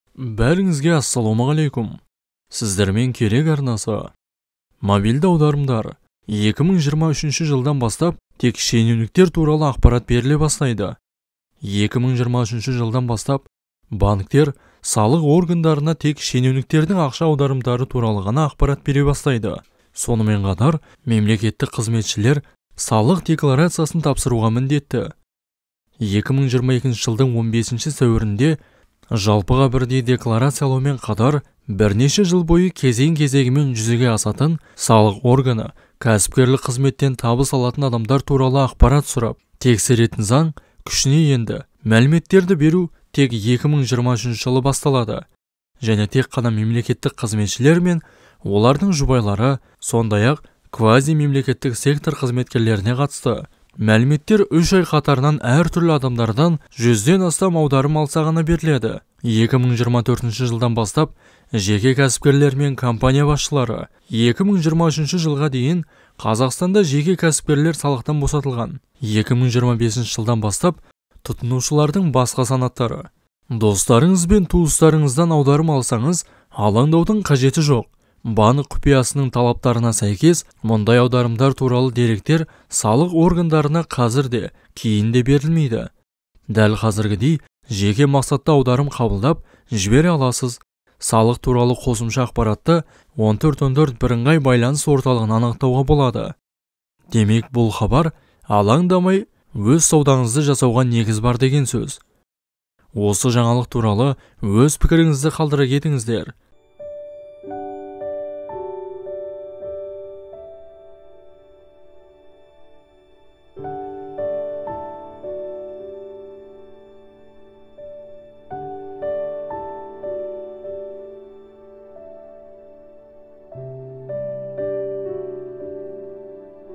СҮЙІНШІ ХАБАР КЕЛДІ, Енді Каспи Переводты Ешкім Тексермейді, Заң Күшіне енді, Керек арнасы
Арнаға қолдау жасағыңыз келсе:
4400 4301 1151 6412

 Бәріңізге рақмет!